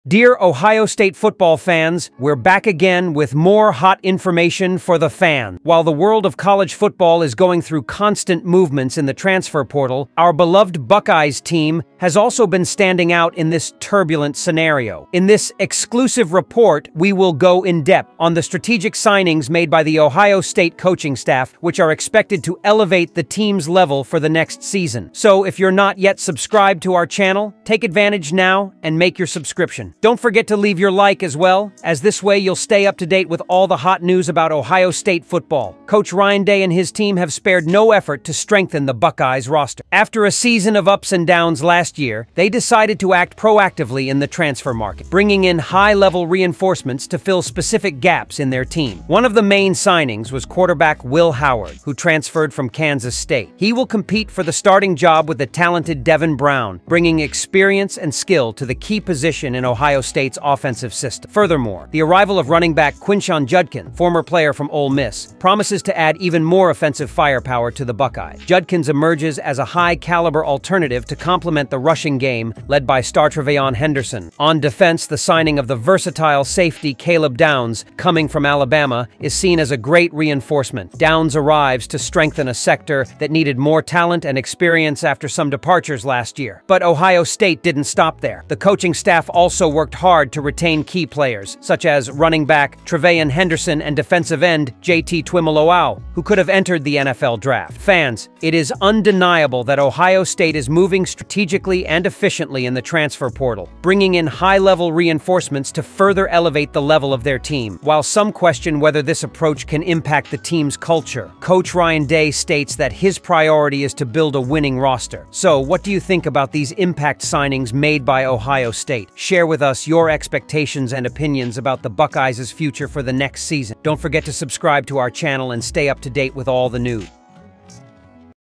URGENT NOW!OHIO STATE STRENGTHENS YOUR TEAM WITH IMPACT REINFORCEMENTS!OHIO STATE FOOTBALL NEWS
URGENT NOW!OHIO STATE STRENGTHENS YOUR TEAM WITH IMPACT REINFORCEMENTS!OHIO STATE FOOTBALL NEWS TODAY

Dear Ohio State Football fans, WE'RE BACK AGAIN WITH MORE HOT INFORMATION FOR THE FANS! While the world of college football is going through constant movements in the transfer portal, our beloved Buckeyes team has also been standing out in this turbulent scenario.
In this exclusive report, we will GO IN DEPTH on the strategic signings made by the Ohio State coaching staff, which are expected to elevate the team's level for the next season. Don't forget to leave your like as well, as this way you'll stay up to date with all the hot news about Ohio State Football!
Coach Ryan Day and his team have spared no effort to strengthen the Buckeyes' roster. After a season of ups and downs last year, they decided to act proactively in the transfer market, bringing in high-level reinforcements to fill specific gaps in their team.
One of the main signings was quarterback Will Howard, who transferred from Kansas State. He will compete for the starting job with the talented Devin Brown, bringing experience and skill to the key position in Ohio State's offensive system.
Furthermore, the arrival of running back Quinshon Judkins, former player from Ole Miss, promises to add even more offensive firepower to the Buckeyes. Judkins emerges as a high-caliber alternative to complement the rushing game led by star TreVeyon Henderson.
On defense, the signing of the versatile safety Caleb Downs, coming from Alabama, is seen as a great reinforcement. Downs arrives to strengthen a sector that needed more talent and experience after some departures last year.
But Ohio State didn't stop there. The coaching staff also worked hard to retain key players, such as running back TreVeyon Henderson and defensive end JT Tuimoloau, who could have entered the NFL draft.